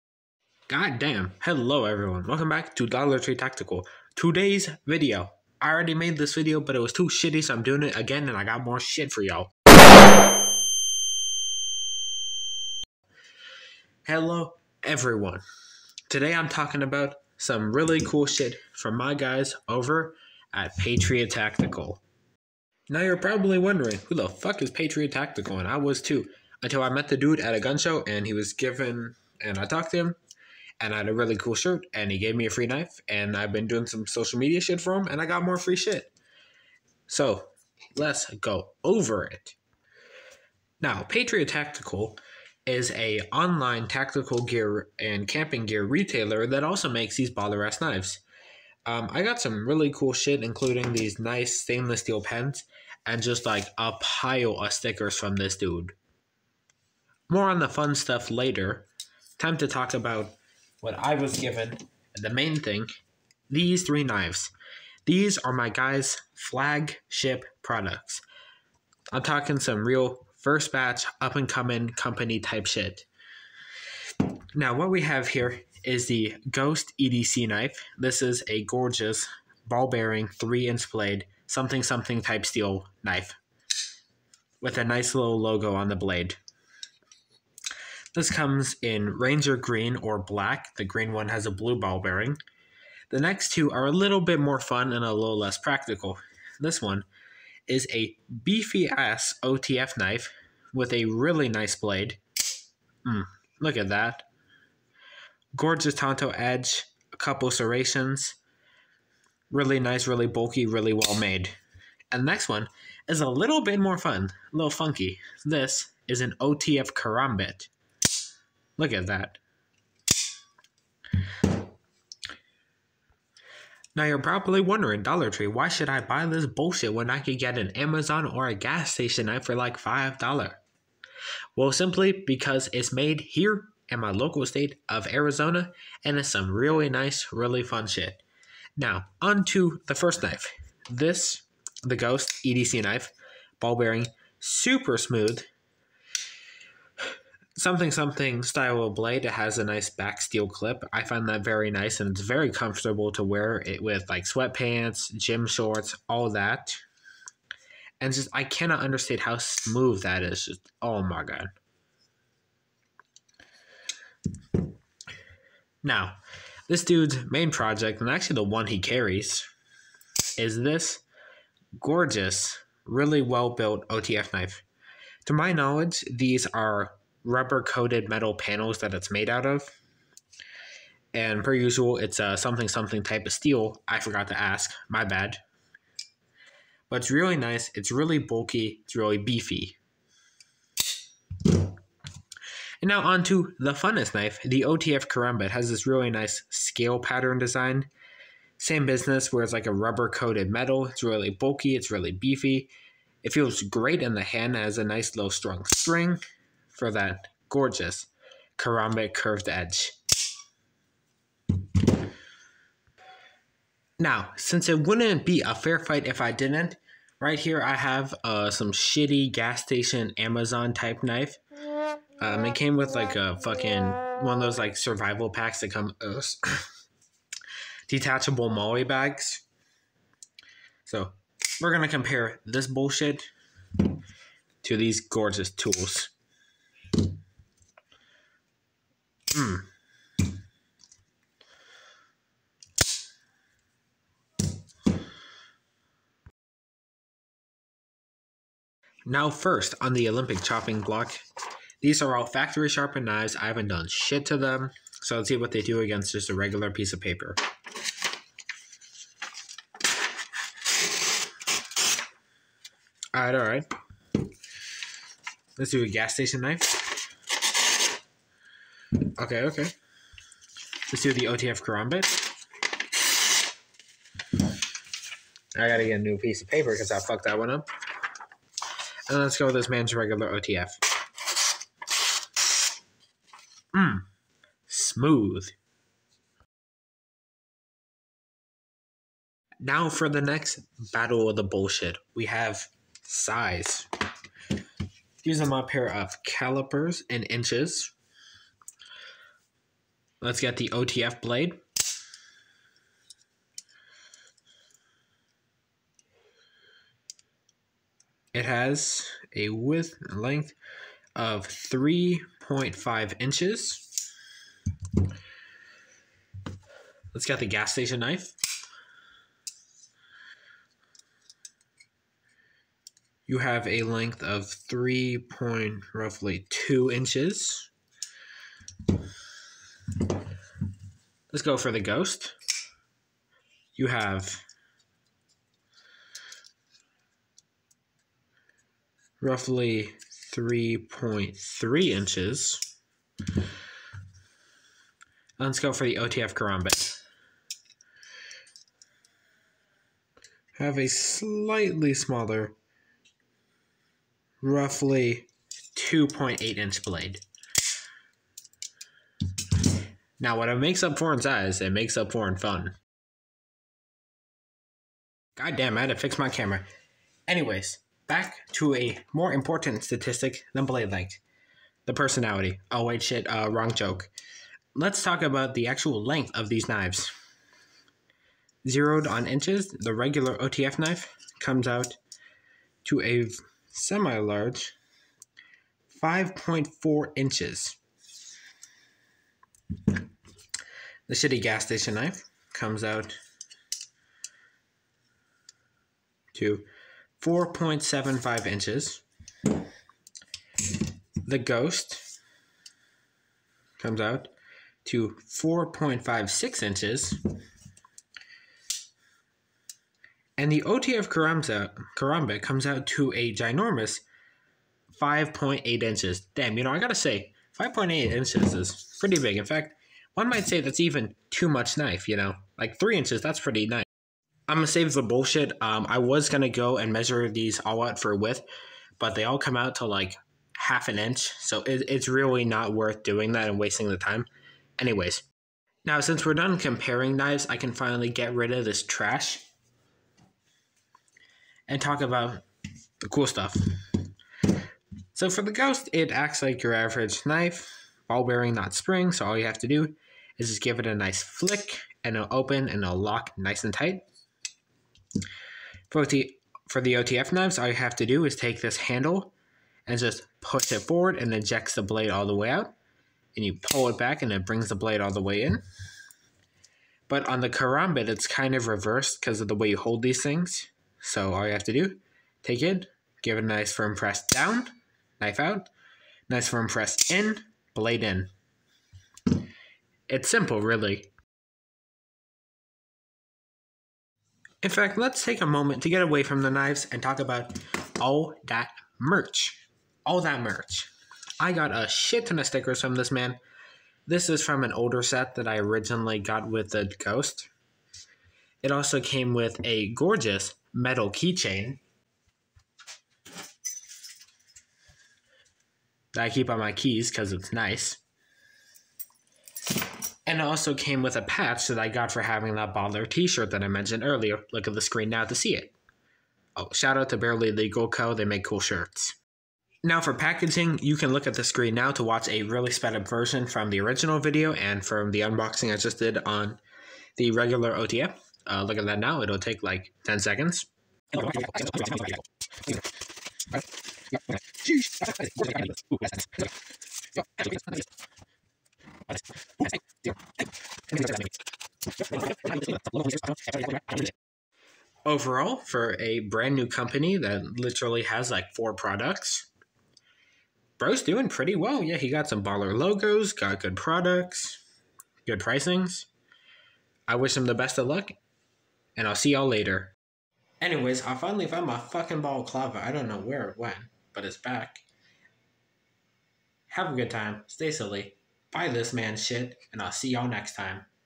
OTF Karambits and Ball-Bearing EDC knives: Patriot Tactical Knife review
My review and unboxing of brand new products from Patriot Tactical a new knife manufacturer and sporting goods dealer with their OTF karambit, OTF knife, and Ghost EDC knife.
Time Codes
00:00 - intro
1:12 - Product/Company background
2:02 - Product Overview
2:18 - Shameless Marketing
4:05 - Review Begins
4:45 - Sharpness Test
6:03 - Blade Length
7:15 - Body Length
8:00 - Quick User Guide
8:39 - Shameless Merch Talk
9:03 - Really Sped Up Unboxing
9:19 - Final Thoughts
9:32 - End Slide